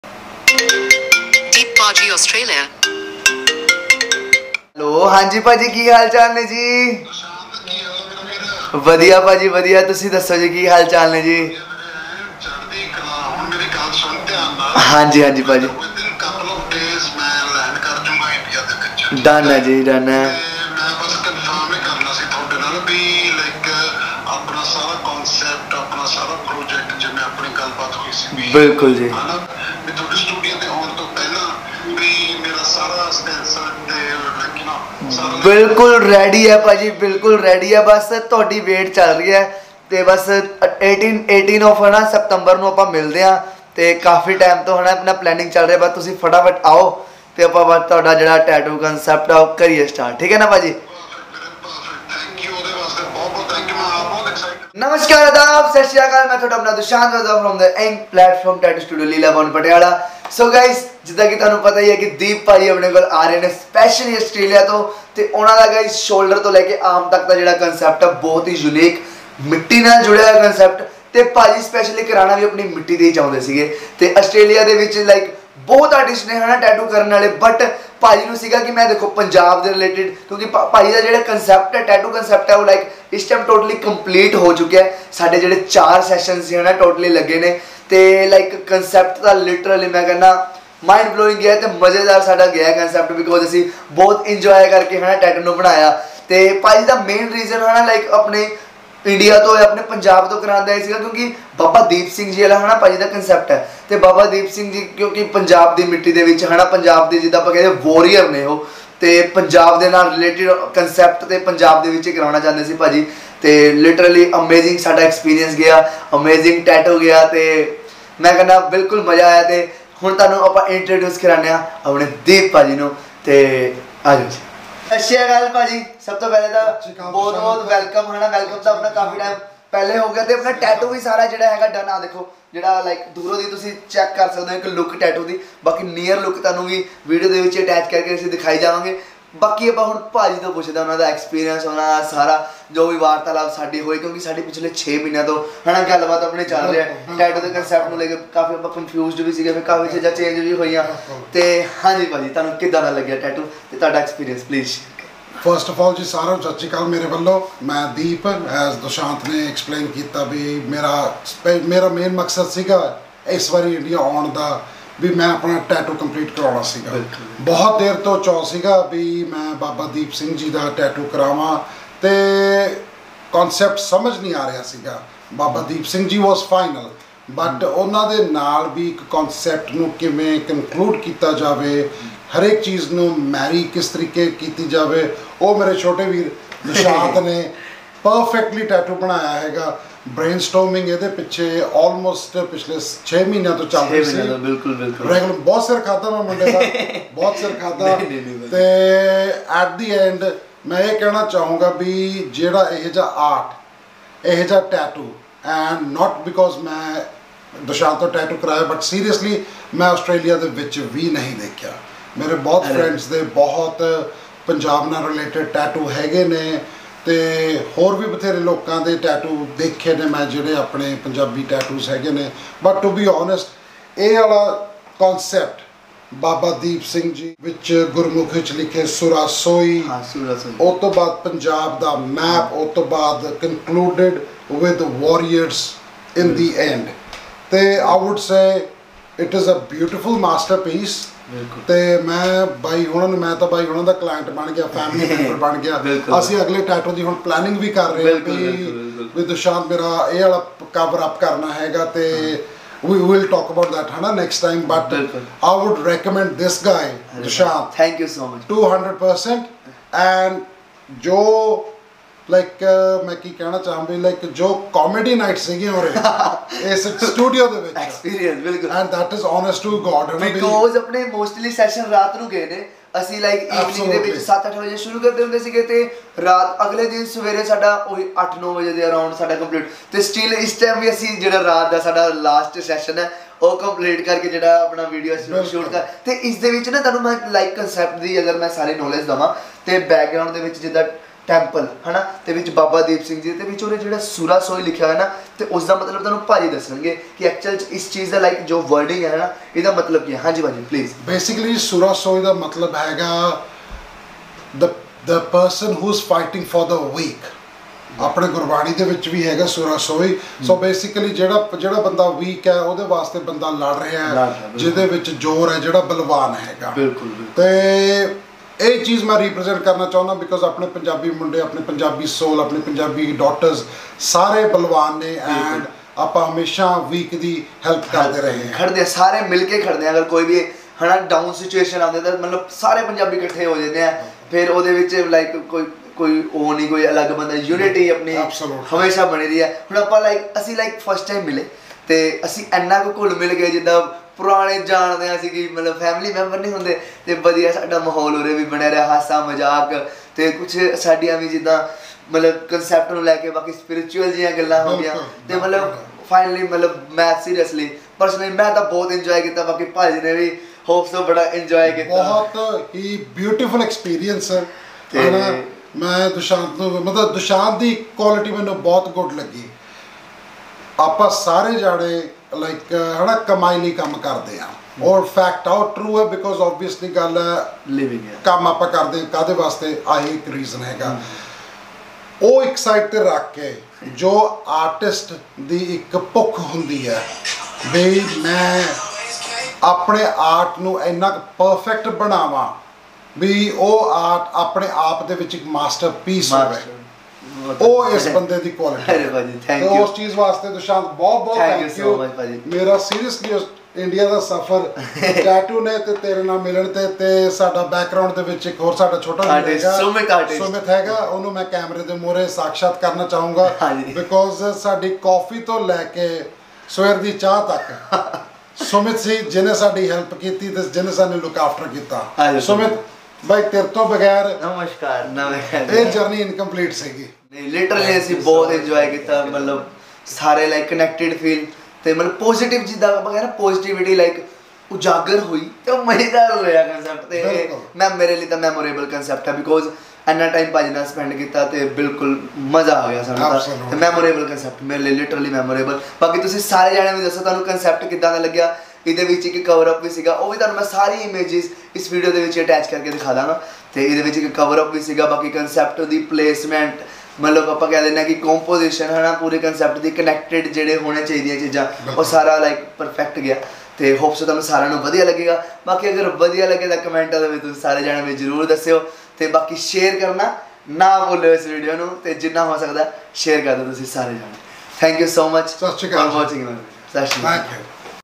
An Australian Man Got Inked In Punjab 🔥😲
.Baba deep singh ji tattoo.
.Full arm tattoo.
.Baba deep singh ji.

💫Hope so Thonu sarya nu eh review te tattoo deep paji da vadia lagya houga😍😍😍 Agar vadia laggya ta jrur like krio,

And Give Your Valuable Reviews in Comments